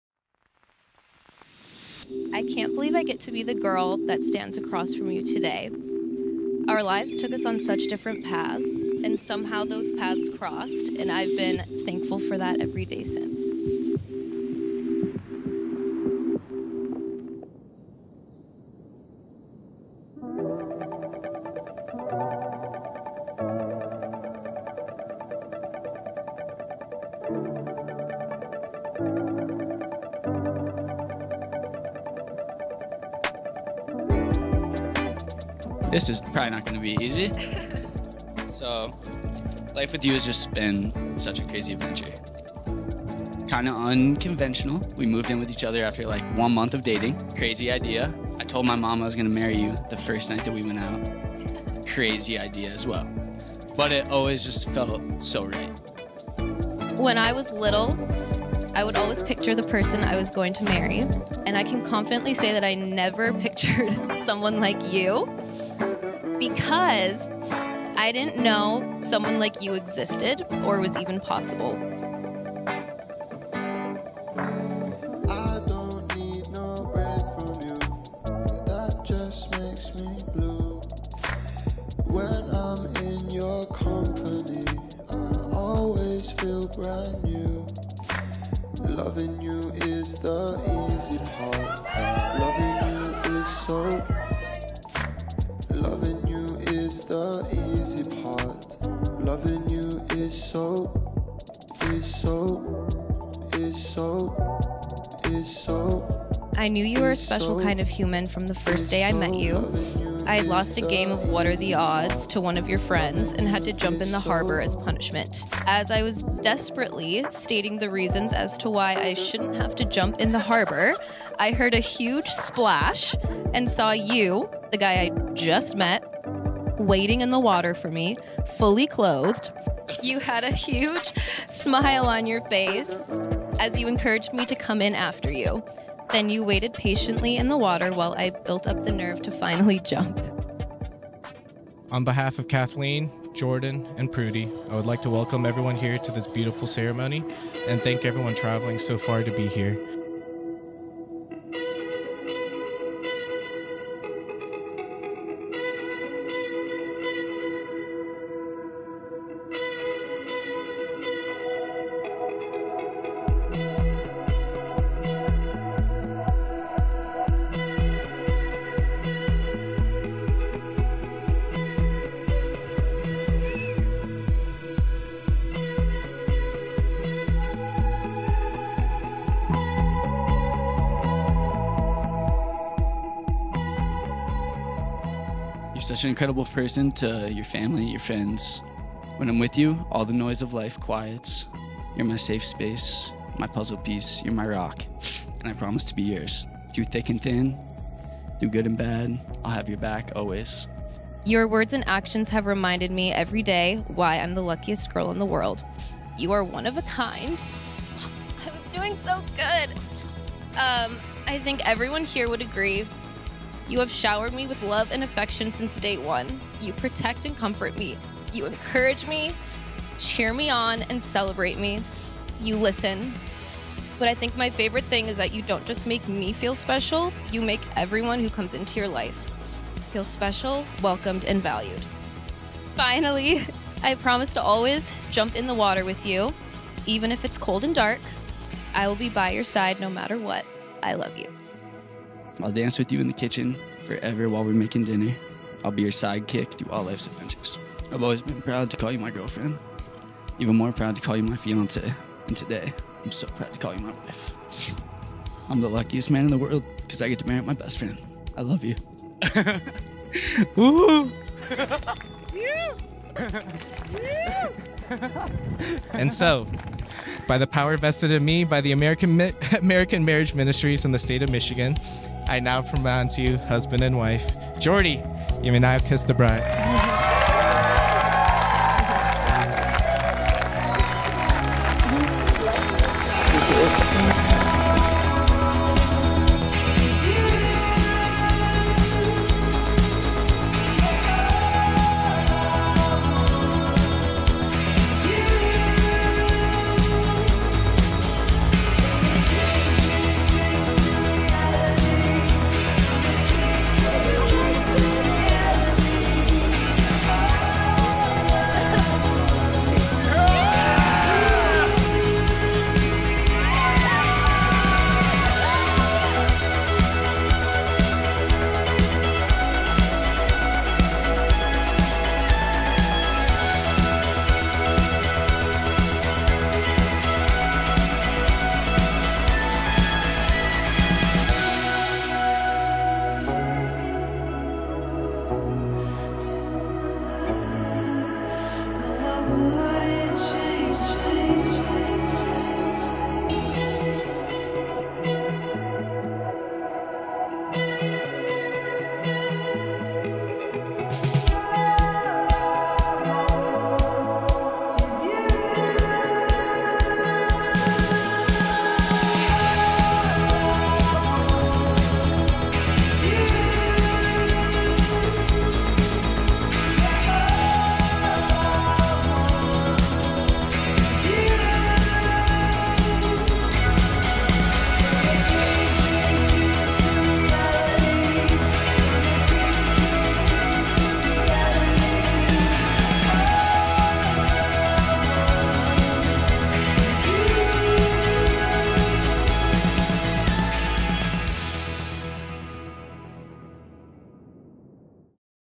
Kathleen and Jordon | Paint River Landing | Wedding Highlight Film
We are Matthew Hill Creative, a Wedding Filmmaking company based in Marquette, Michigan. You can see more of our work

Kathleen and Jordon had an absolutely stunning wedding day. Their energy and excitement was so fun, and it was felt by every guest. Their ceremony centered around their personally written vows, which summed up their love story so well.

The venue was right on the Paint River and was the most ideal spot for them. Kathleen grew up just down the river, and it was so special that they were able to have their wedding in her hometown.

It was a joy to be a part of this day!